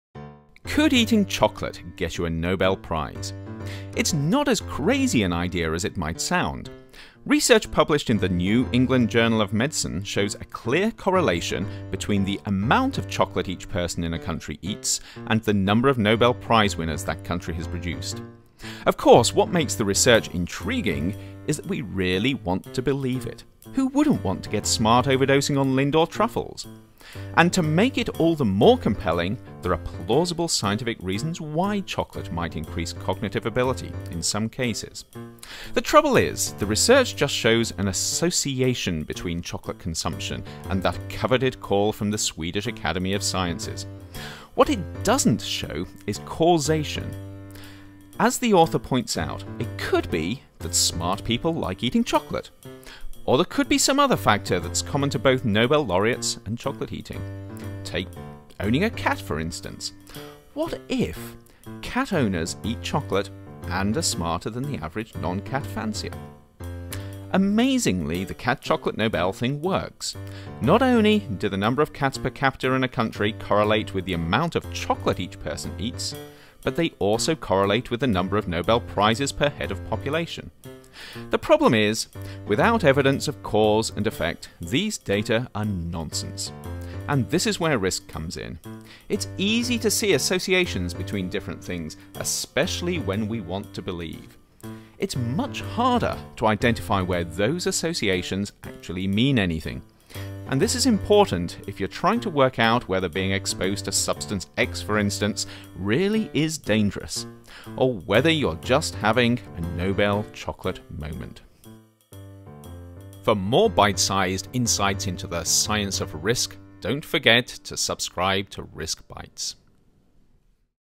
هل من الممكن أن يؤدي الشوكولا للحصول على جائزة نوبل؟
ترجمة: مروة بكداش

في حال عدم ظهور الترجمة يرجى الضغط على CC التي تظهر على اليمين واختيار اللغة العربية
الكيمياء العربي | حيث يبدأ التفاعل